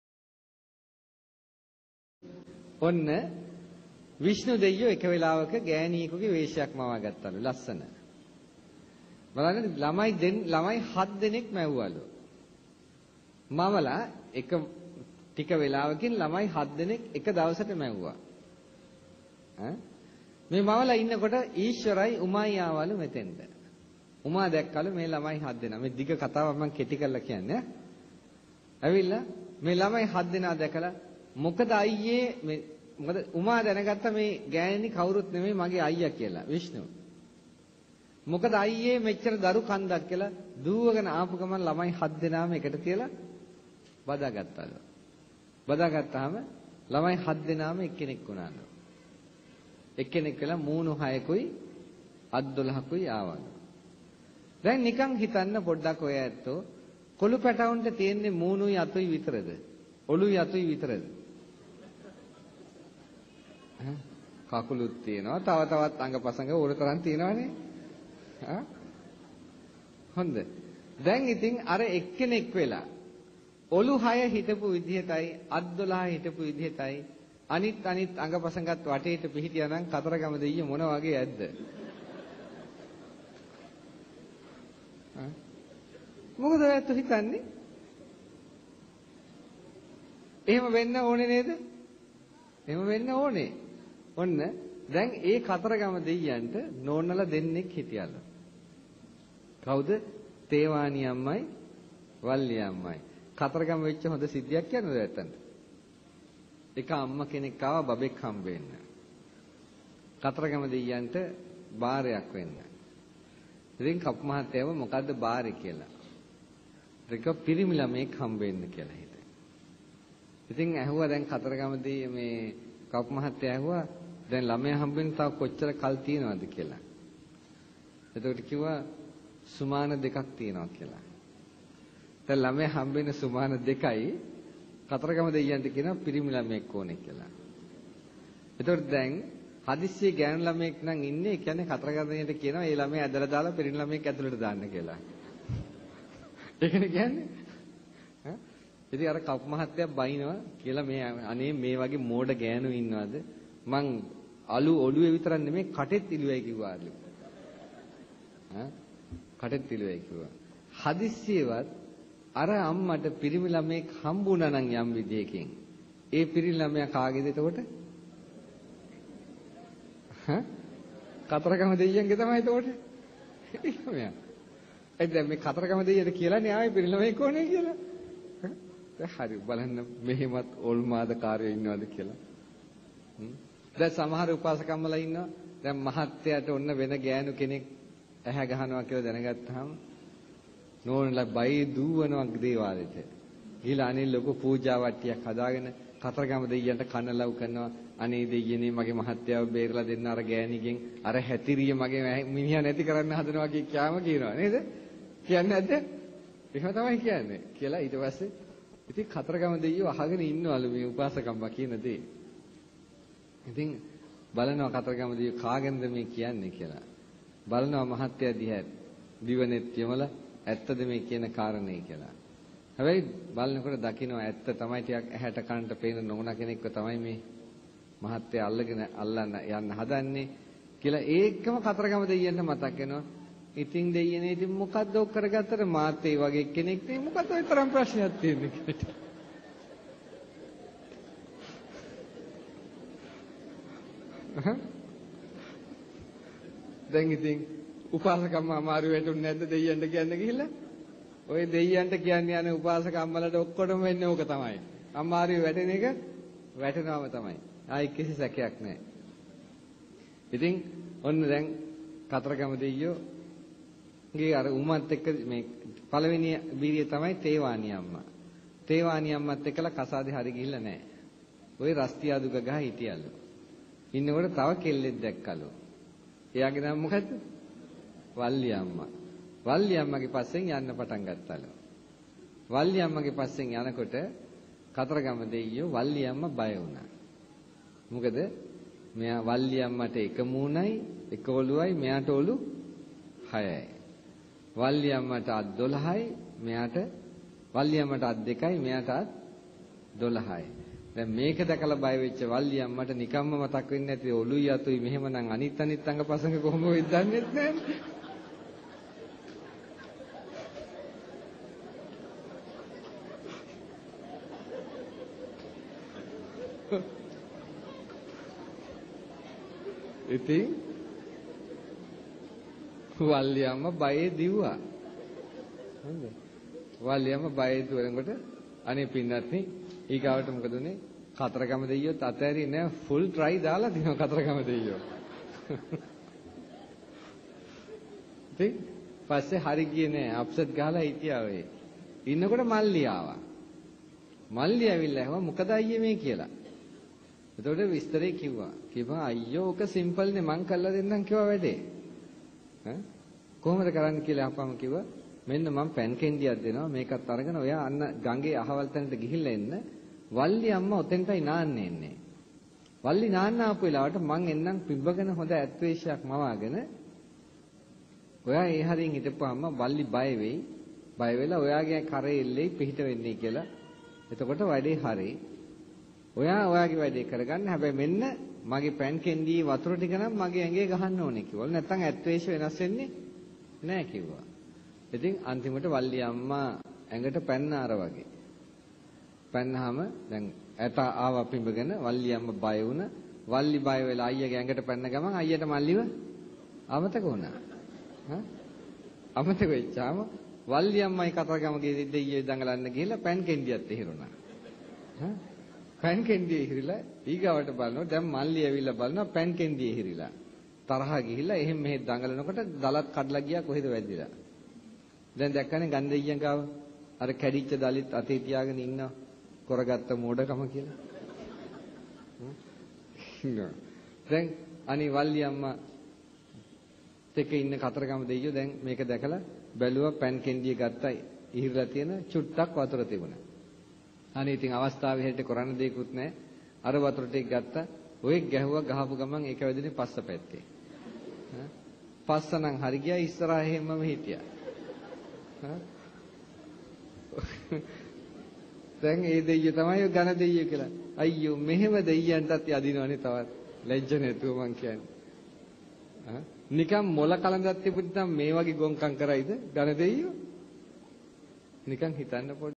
කතරගම දෙවි කථාව | ගංගොඩවිල සෝම හිමි බණ | Katharagama deviyo | Ven. Gangodavila Soma himi bana
නමෝ බුද්ධාය !
බුද්ධ සාසනං චිරං තිට්ඨතු !